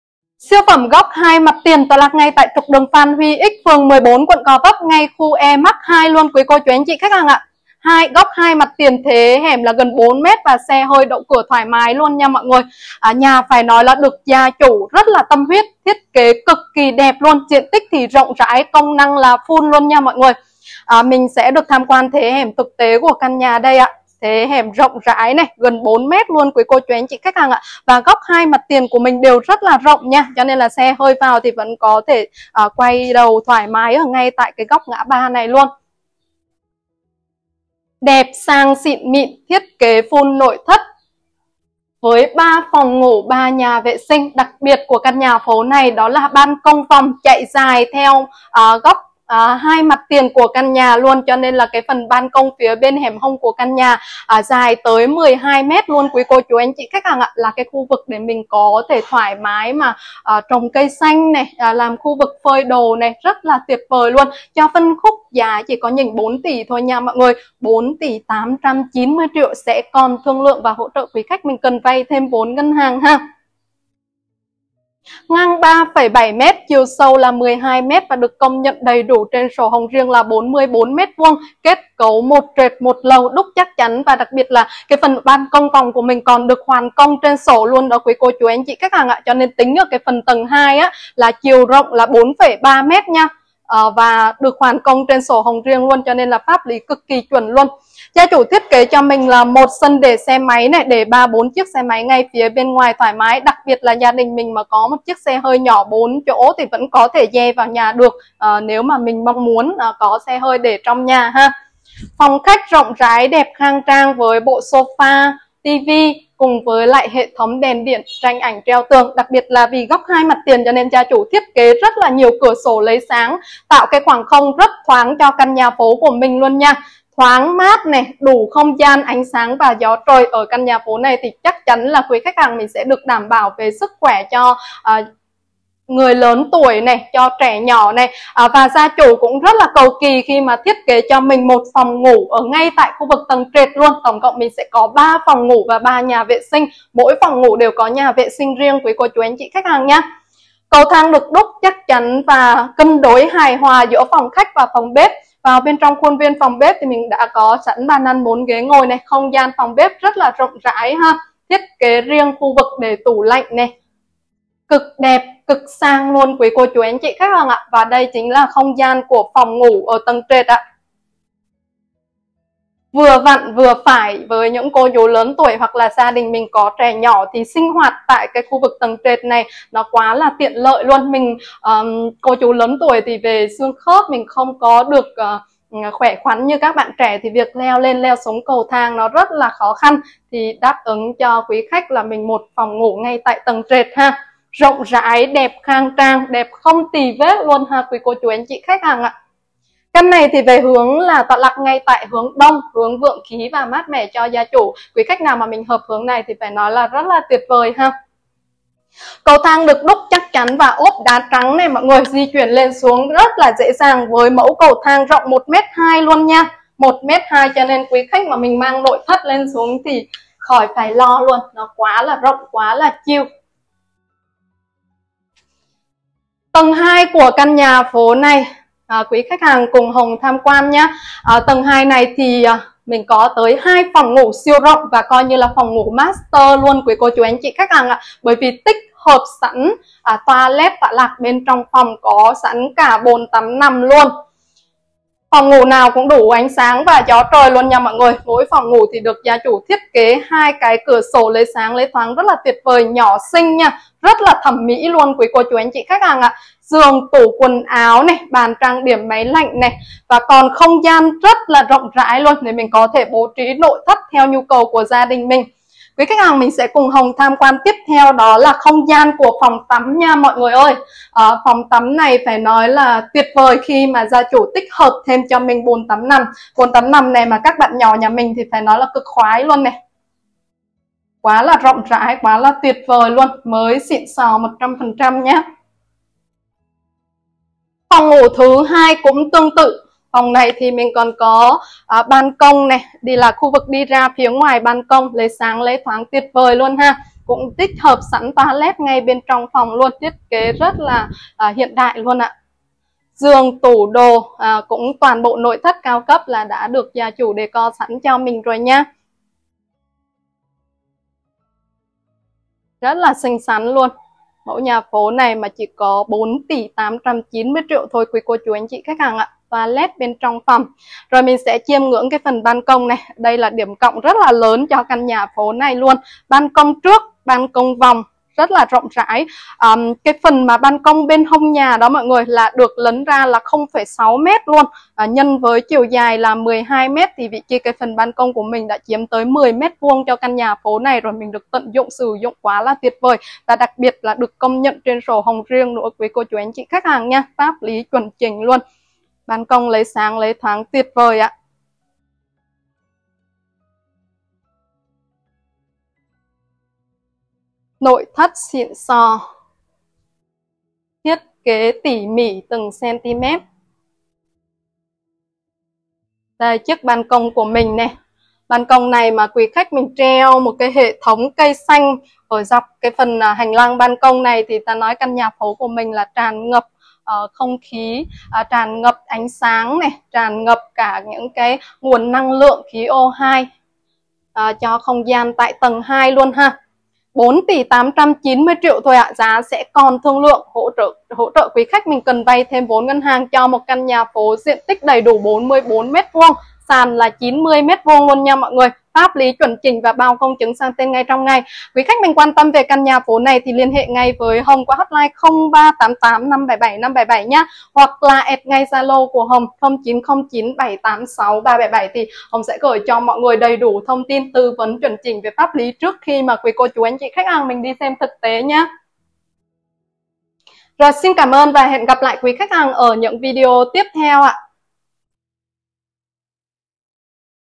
BÁN NHÀ 2mặt HẺM XE HƠI 4M khu Emart2 DTCN 44m2 3phòng ngủ 331 Phan Huy Ích P14,Gò Vấp GIÁ 4.89TỶ TL
BÁN NHÀ 2 mặt HẺM XE HƠI 4M khu Emart2 DTCN 44m2 3phòng ngủ
- 331/51/78 Phan Huy Ích P14, Gò Vấp
- GIÁ 4.89TỶ TL